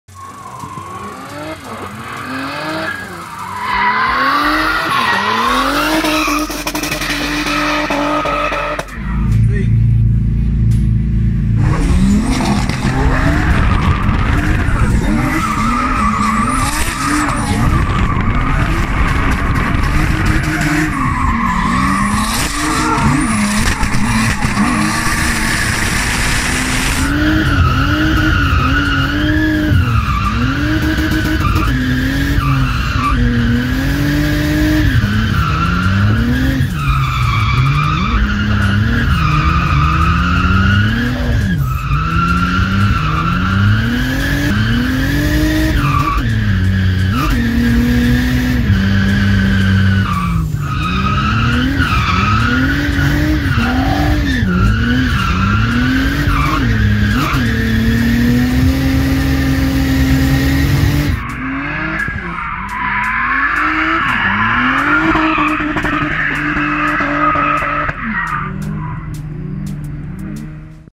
Tor Jastrząb l ECULAB l rusEFI
Driver: Krystian Gruszczyński
Car: BMW E36 M50B25 Turbo HX35
Track: Tor Jastrząb Poland

BB_V3 ECU by ECULAB
rusEFI firmware